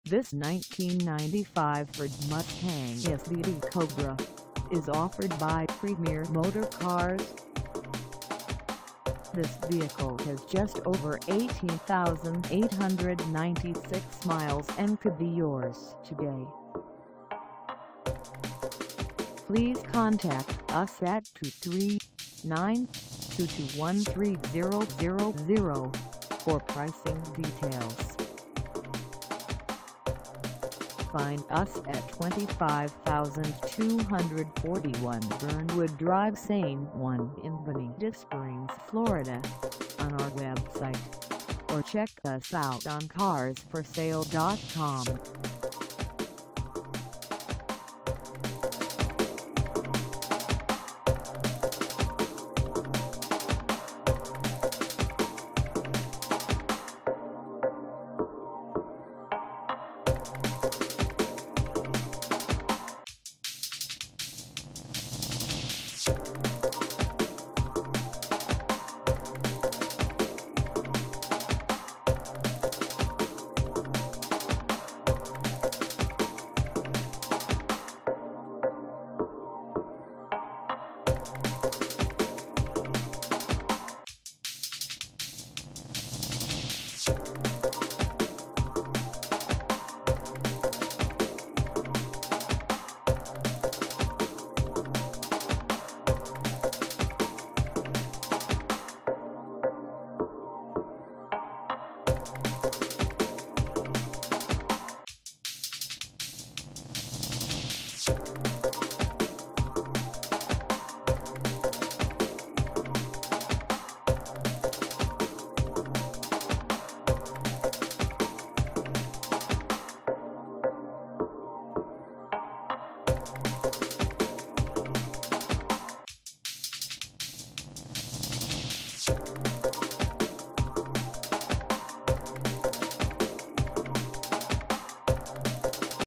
1995 Ford Mustang SVT Cobra for sale in Bonita Springs, FL 3
This 1995 Ford Mustang SVT Cobra is for sale in Bonita Springs, FL 34135 at Premier Motorcars.
Engine: V8 5.0L V8
Transmission: Automatic 4-Speed
Color: Black
Mileage: 18896
Contact Premier Motorcars 25241 Bernwood Dr. Ste 1 Bonita Springs, FL 34135 to test drive this 1995 Ford Mustang SVT Cobra  today.